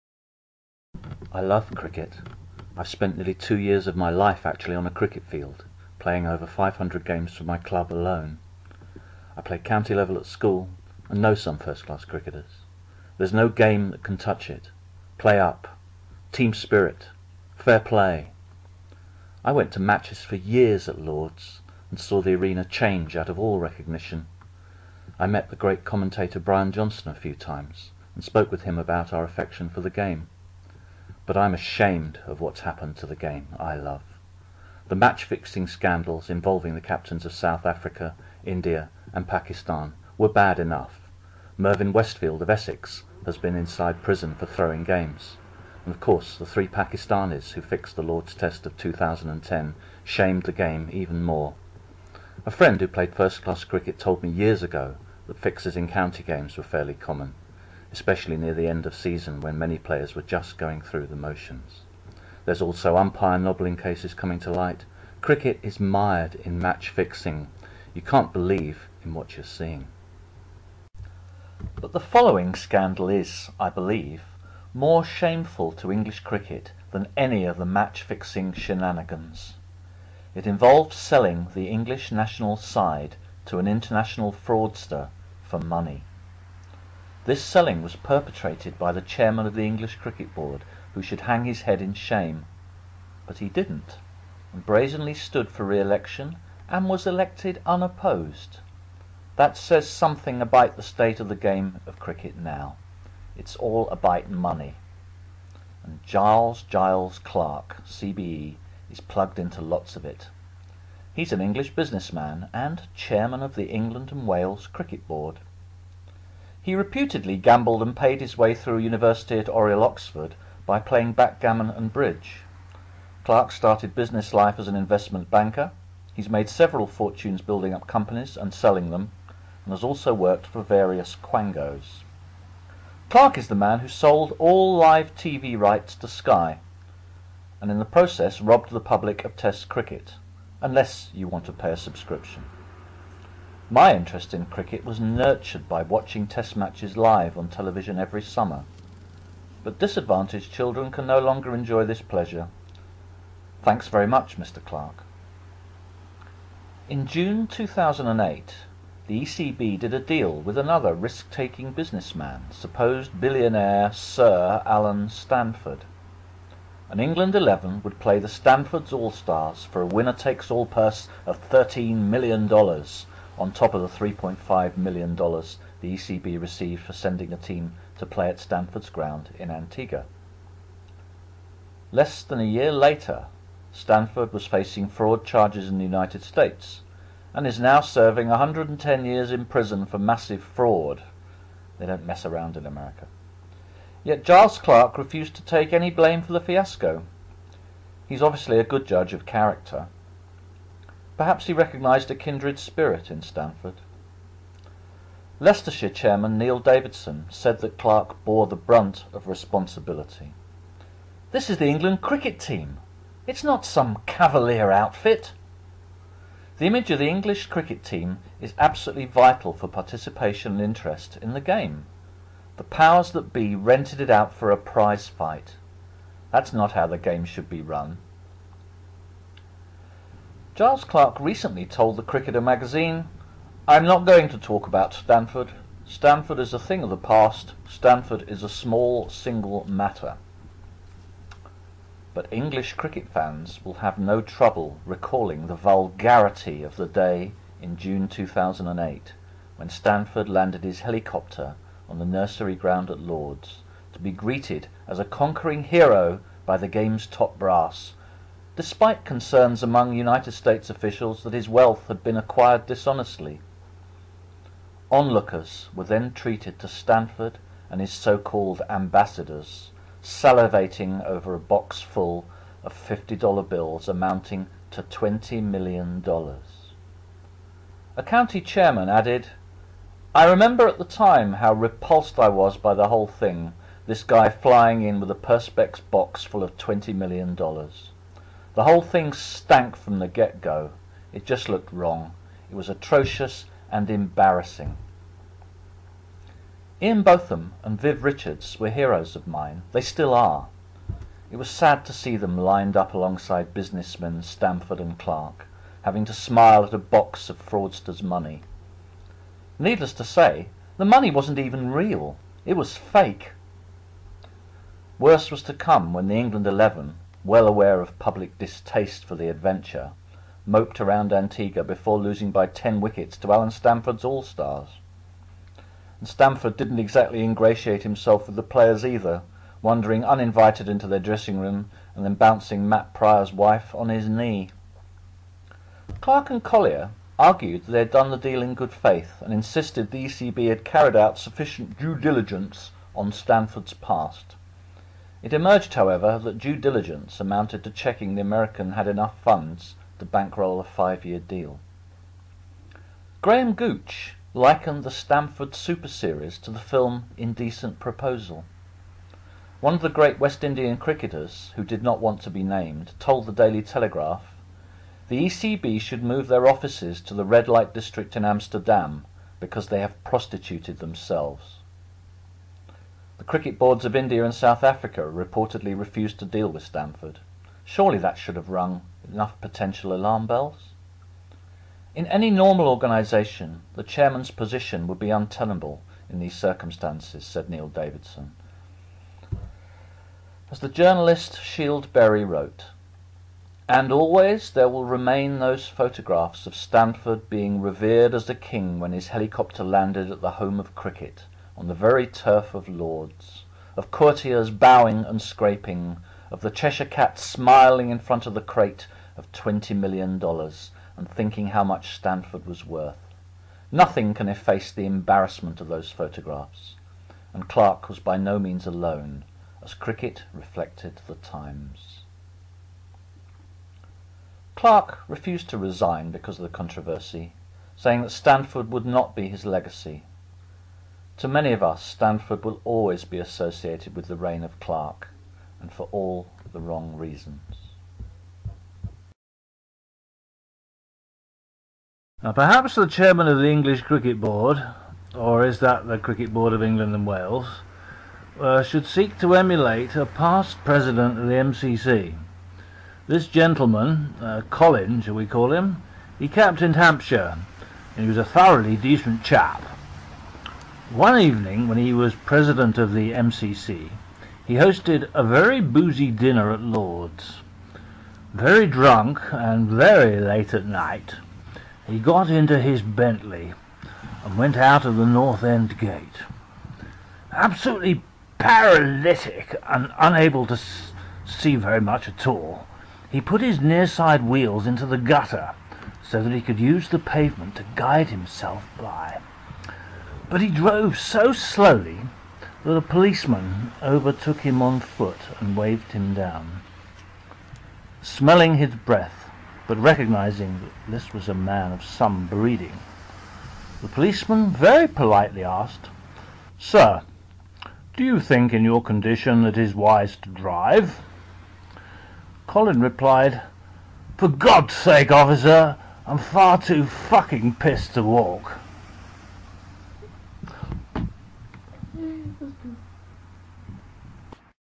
Cricket's Shame, Giles Clarke and the FRAUDSTER
This film tells the story of how Chairman of the Cricket Board of England and Wales did a deal with Allen Stanford, who was later imprisoned for 110 years for fraud. It was shameful, tasteless, and cheap, but Giles Clarke, did not resign.